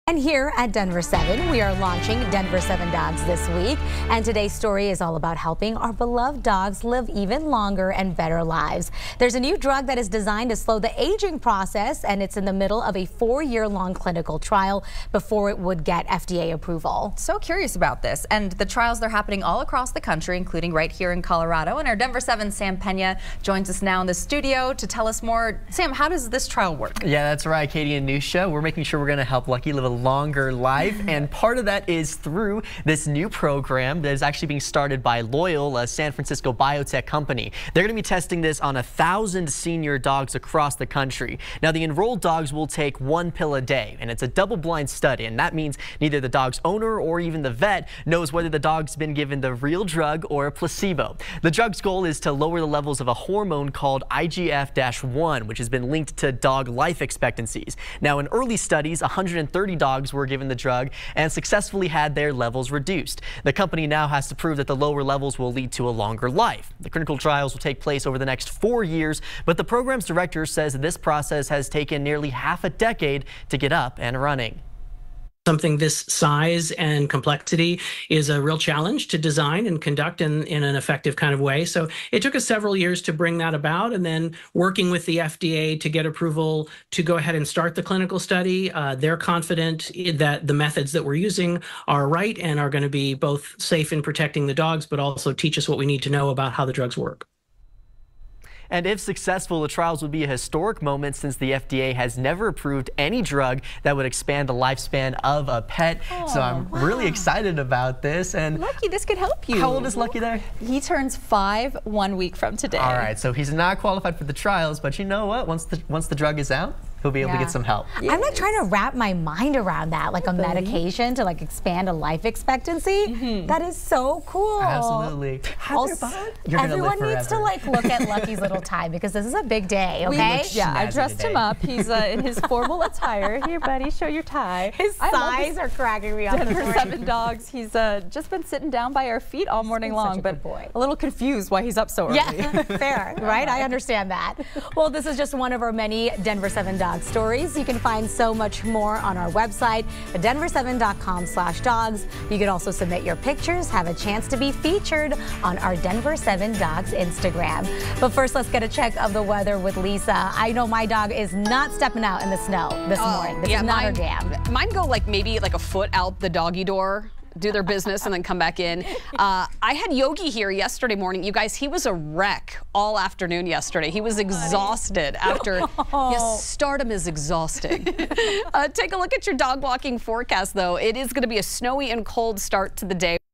Clinical trials underway for new drug that could extend CO dog's lifespan
Clinical trials are underway in Colorado for a new drug that could extend a dog's lifespan. Loyal, a San Francisco based biotech company, is developing the drug and seeking FDA approval.